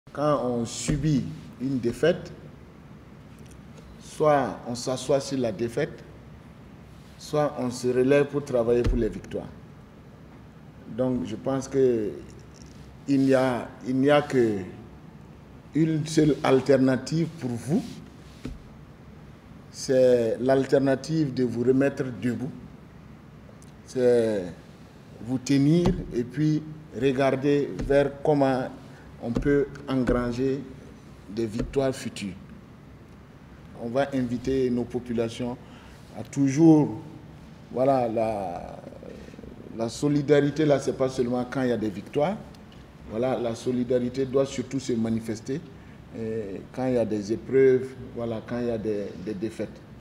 Mot du président du Faso aux Etalons
De retour de la CAN202, les Etalons sont reçus par le Président du Faso.
« Quand on subit une défaite, soit on s'assoit sur la défaite, soit on se relève pour travailler pour les victoires.»
Lieutenant-Colonel Paul Henri Sandaogo Damiba, Président du Faso.